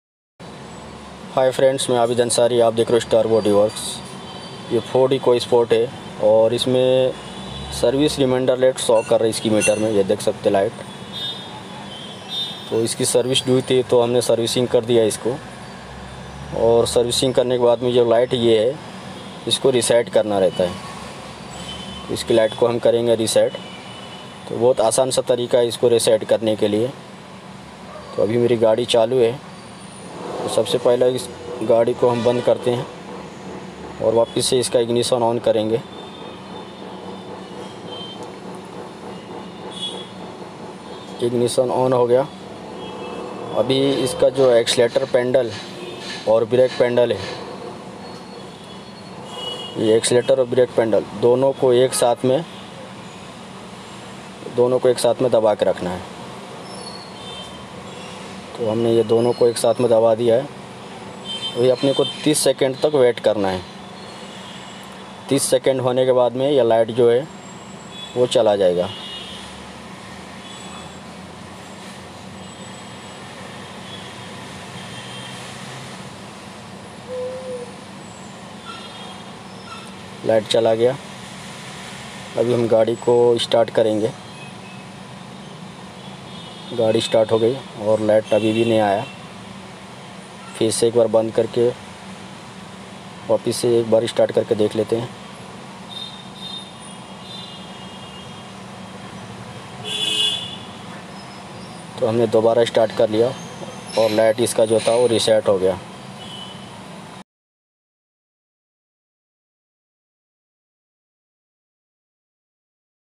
how to reset engine service reminder light | Ford EcoSport सर्विस रिमाइंडर लाइट रिसेट कैसे करें
Ford ecosport engine servicing karne ke bad service reminder light reset karne ka Sahi tarika